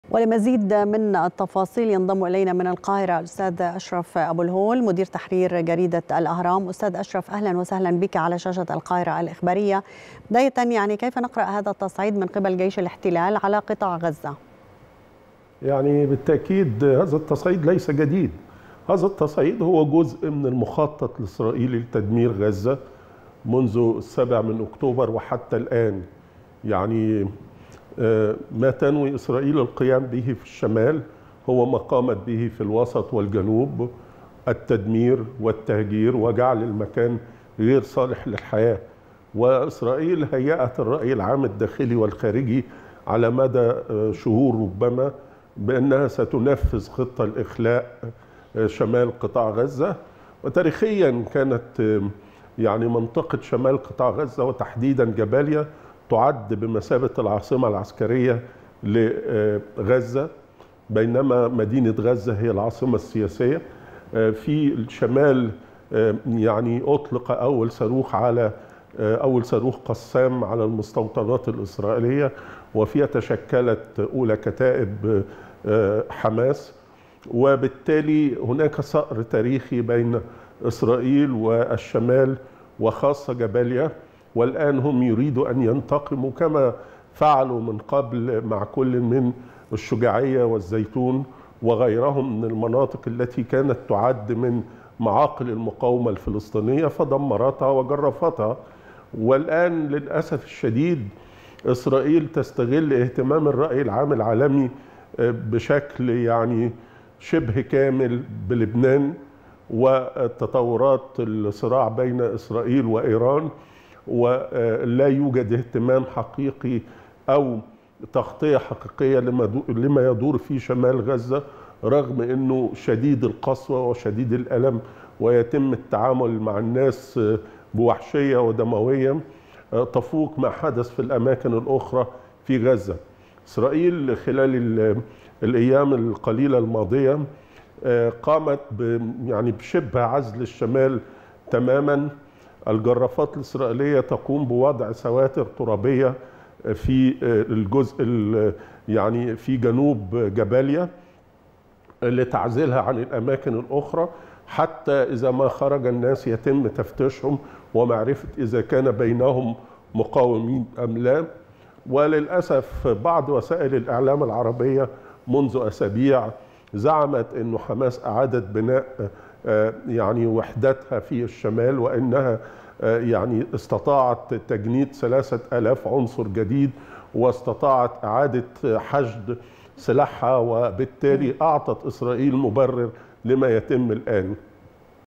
أشرف أبوالهول: هناك ثأر تاريخي بين إسرائيل وشمال غزة خاصة “جباليا” وهذا سبب انتقامهم
أشرف أبوالهول مدير تحرير جريدة الاهرام: هناك ثأر تاريخي بين إسرائيل وشمال غزة خاصة “جباليا” وهذا سبب انتقامهم

14/11/2024